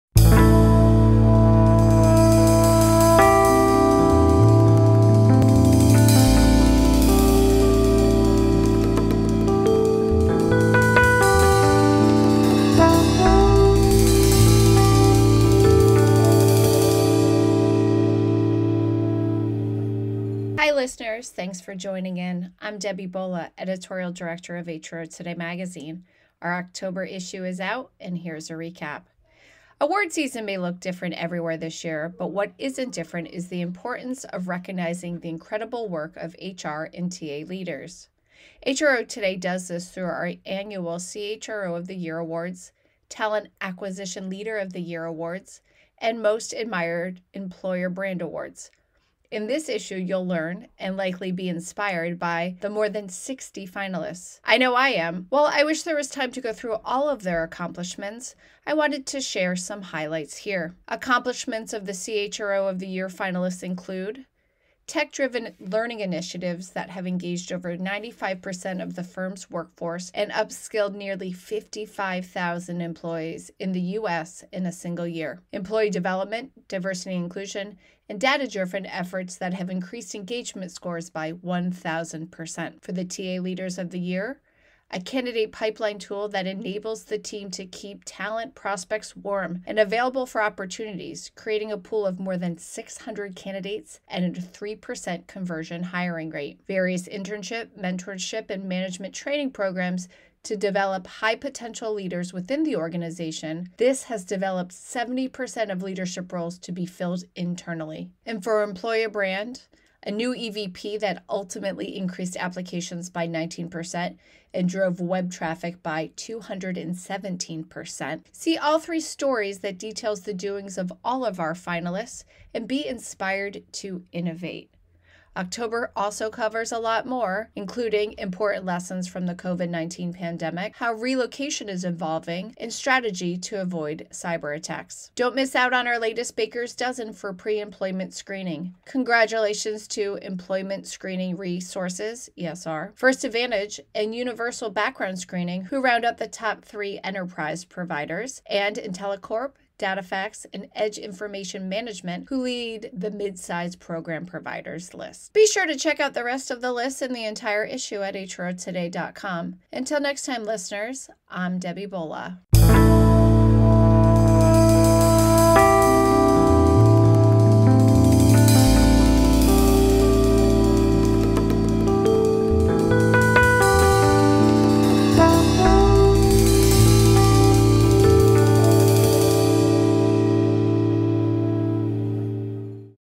HRO Today October 2020 Issue Recap
Editorial Director Debbie Bolla shares an overview of the October issue of HRO Today.